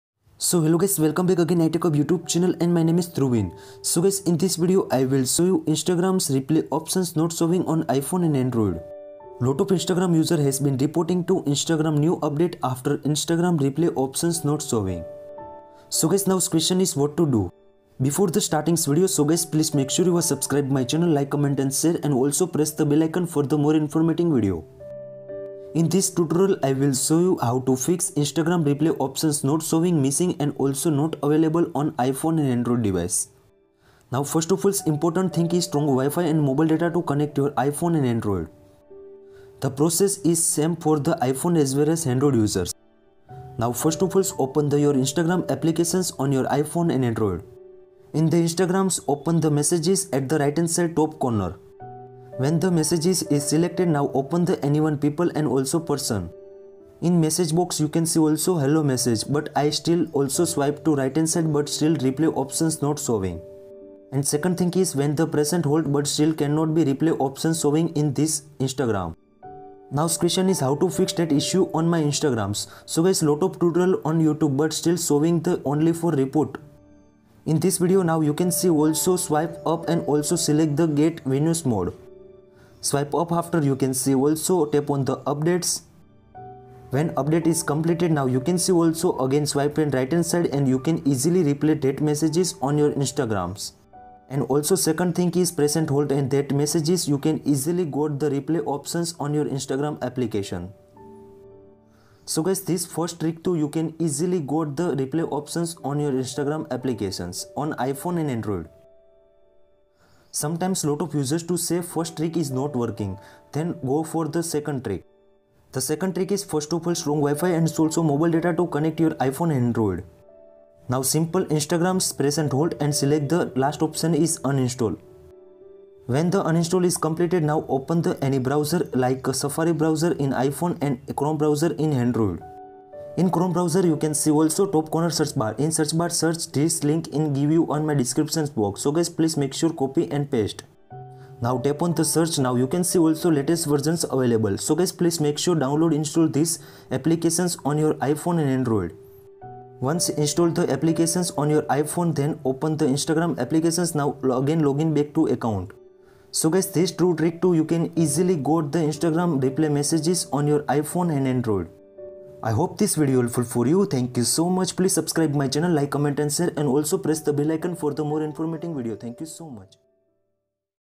Instagram Reply Option Not Showing - [FIXED]
Lot Of Instagram Users Has Been Reported To Instagram Reply Option Not Showing (iPhone & Android)

Best Deals
Boya M1 Mic
iPhone 13
iPhone 12 Pro Max
iPhone 11
Aiprpods Pro
iPhone Charger
Mag-Safe Charger